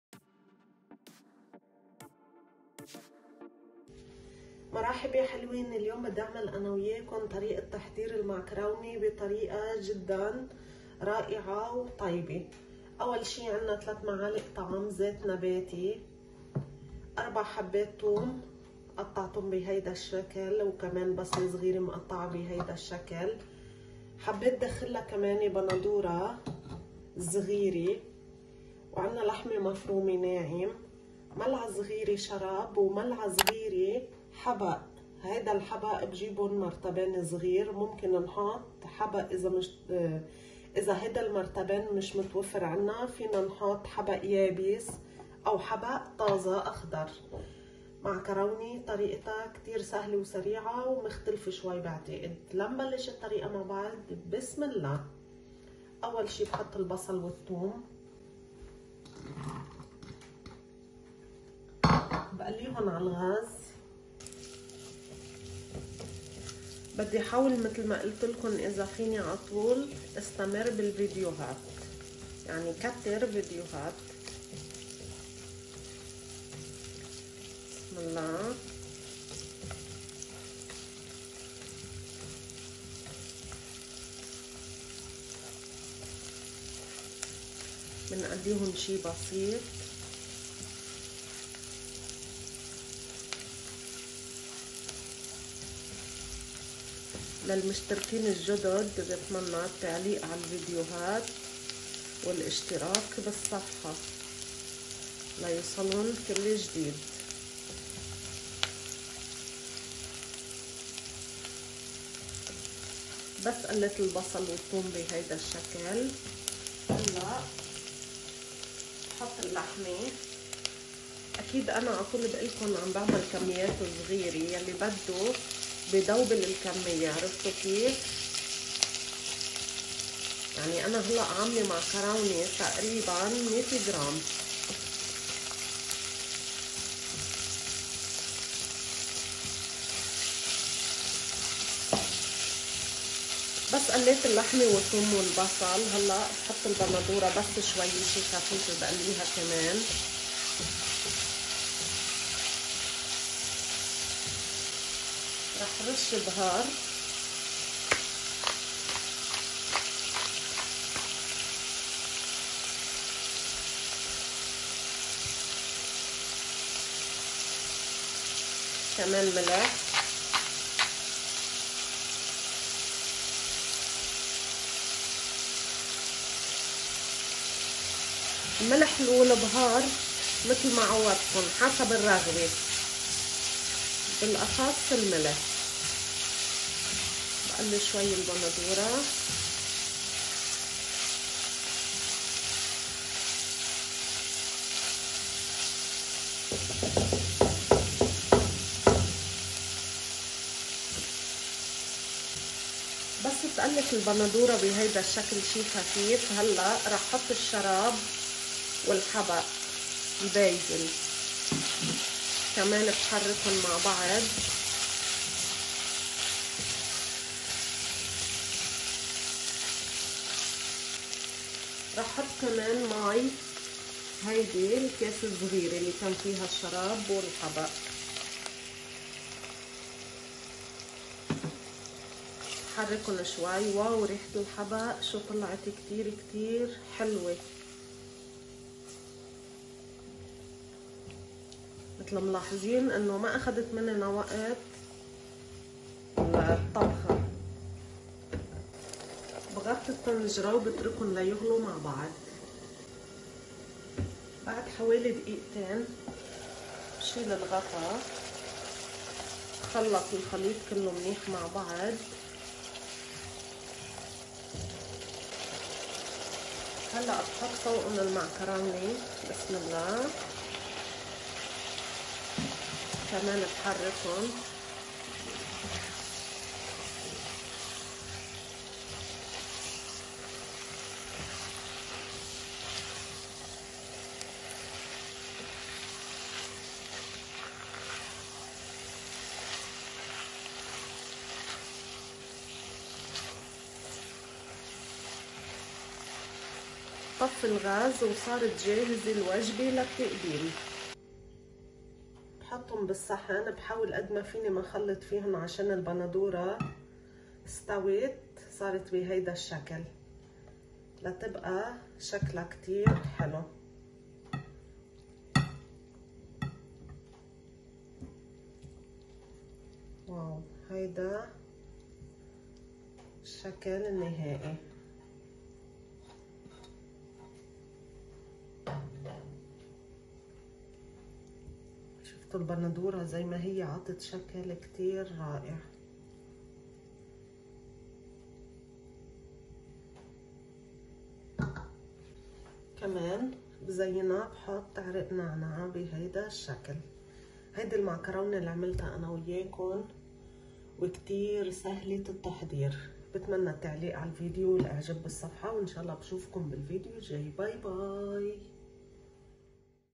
Spaghetti Recipe - سباغيتي معكرونة بالصلصة واللحم المفروم
Ingredients
100 g of spaghetti
4 cloves of garlic
small onion
mincemeat
3 tablespoons of oil
salt
black papers
1 teaspoon of tomato puree
1/2 teaspoon of bazil
tomato